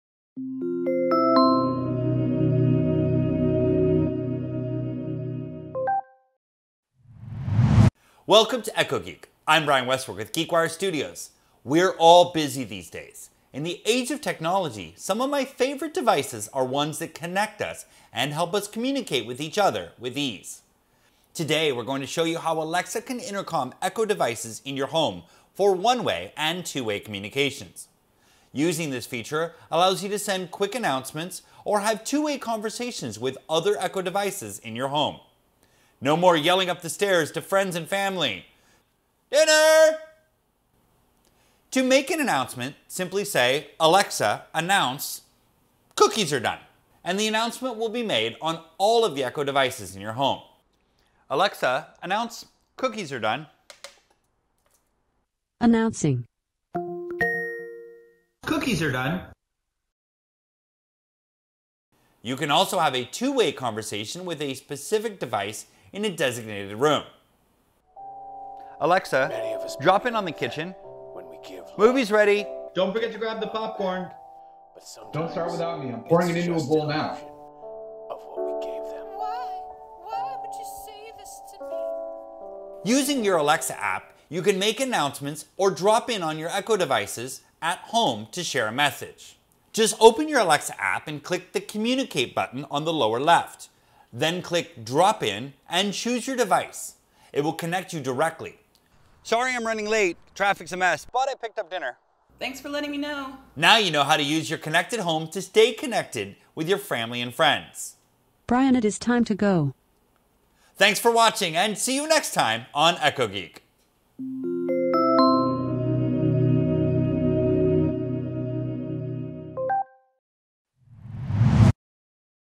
Echo Geek: Using Alexa as an Intercom
Learn how to use Alexa as an intercom on Echo devices in your home for one-way and two way communications.